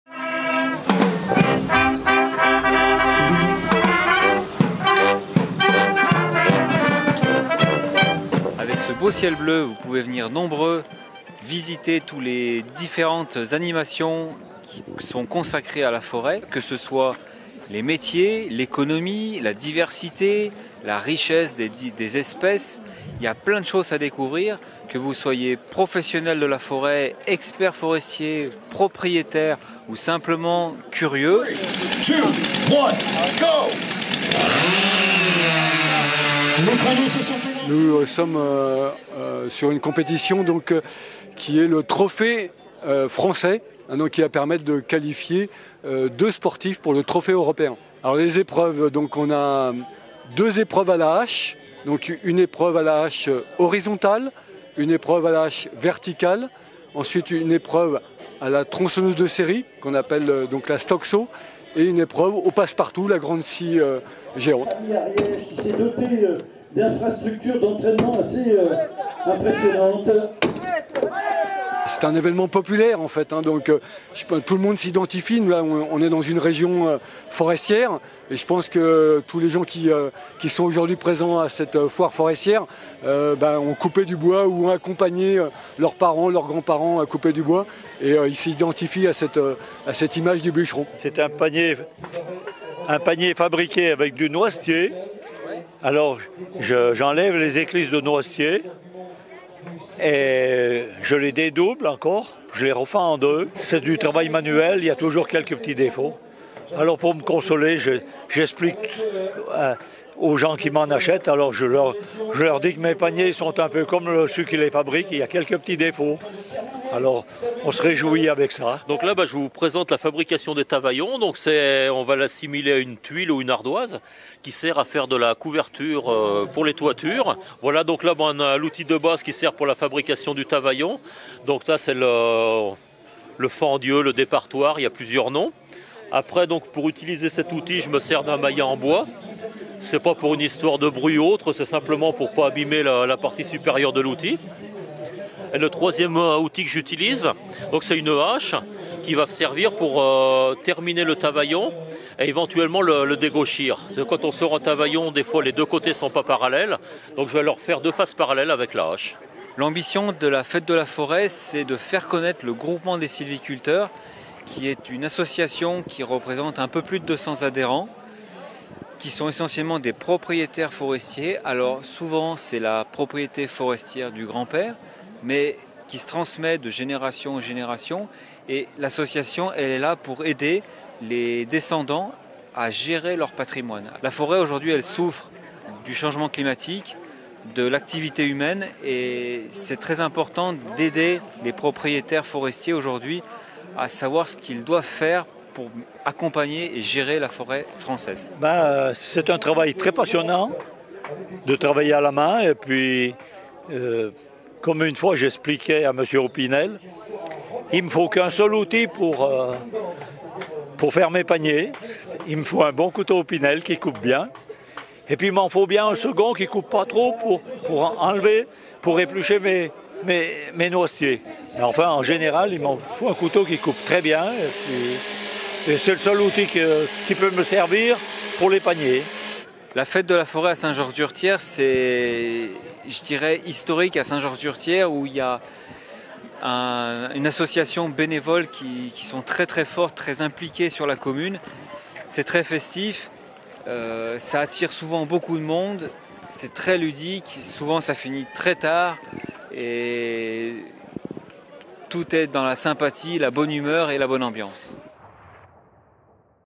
Saint Georges d’Hurtières : La fête de la forêt en Belledonne
LOISIRS - La fête de la forêt en Belledonne🌳

Le groupement des sylviculteurs des Hurtières ont organisé ce week-end, la traditionnelle fête de la forêt en Belledonne, plus de 2 000 personnes ont participé à cette manifestation. Plusieurs animations étaient proposées : démonstrations de bûcheronnage et de broyage, exposition de matériels forestiers, sculptures sur bois…